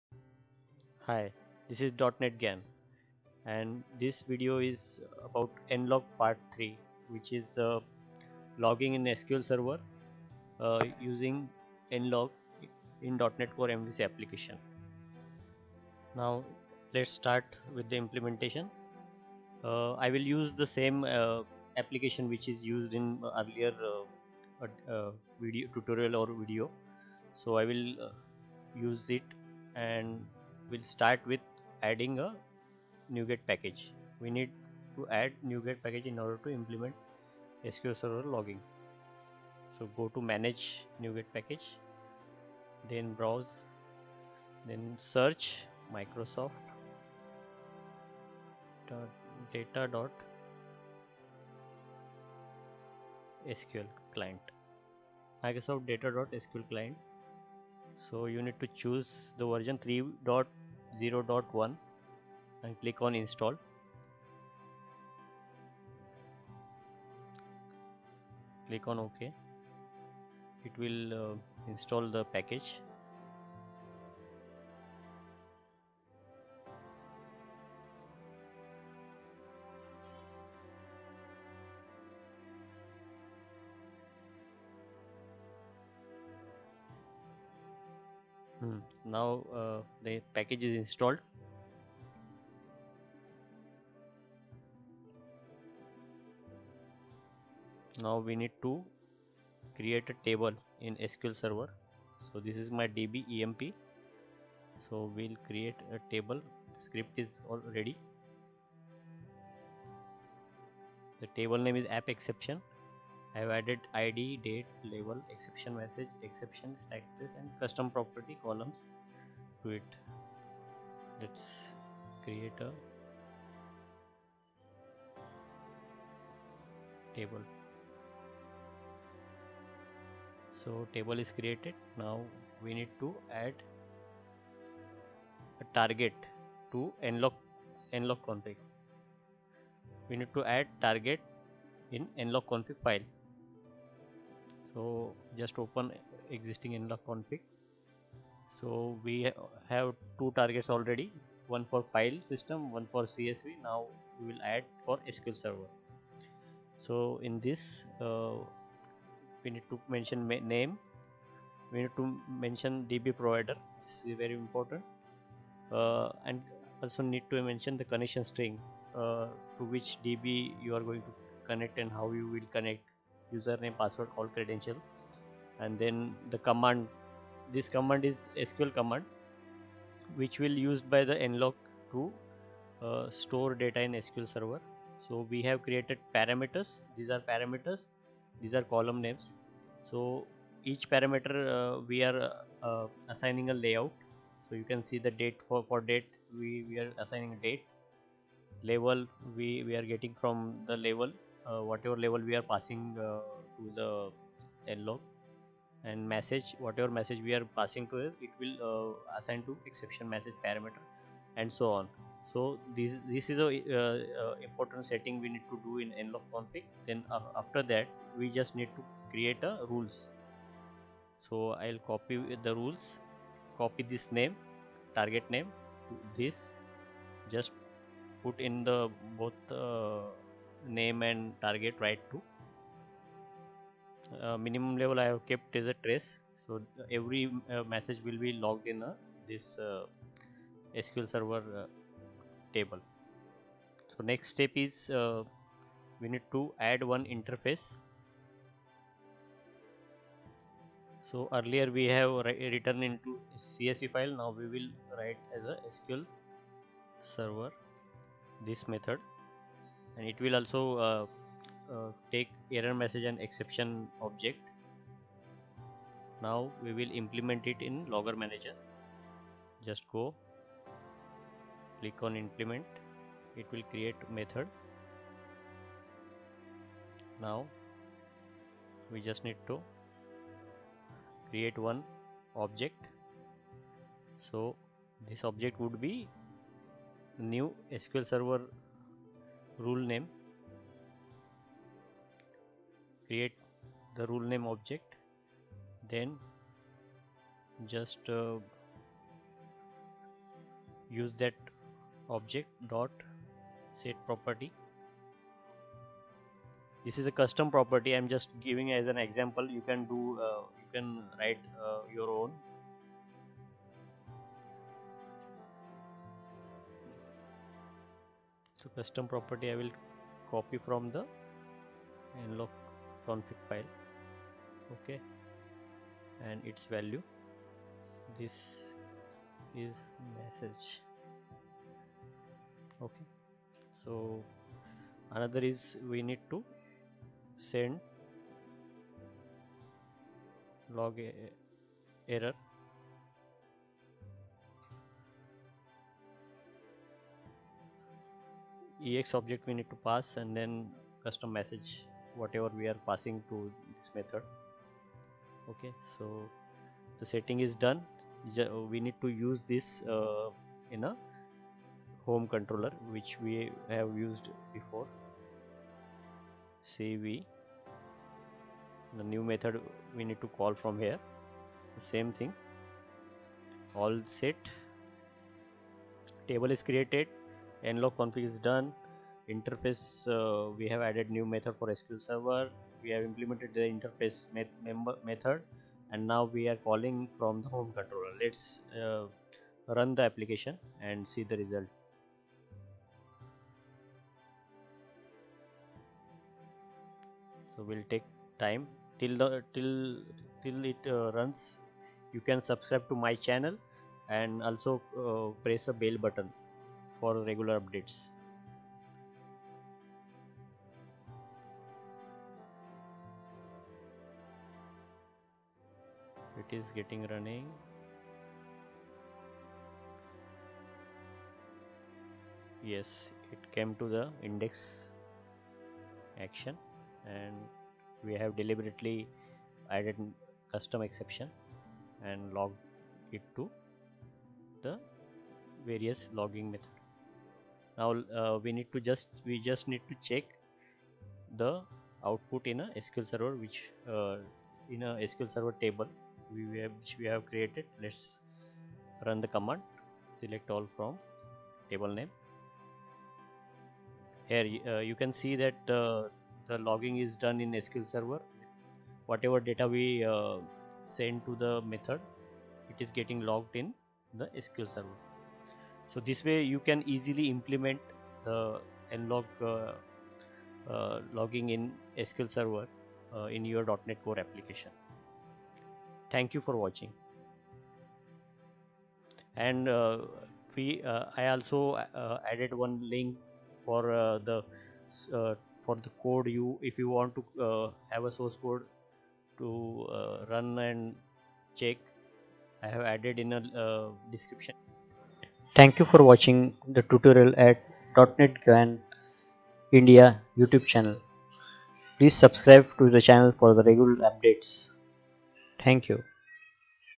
Implement Logging in Sql Server Database using Nlog in .Net Core MVC Application
I will show you how you can store the exception and custom logs in SQL server database using Nlog in .net core MVC application. Storing logs in SQL server database can help us to query the exception data efficiently and we can show it to UI if needed to check exception upfront.

You can find the source code for Implement Logging in Sql Server Database using Nlog in below URL: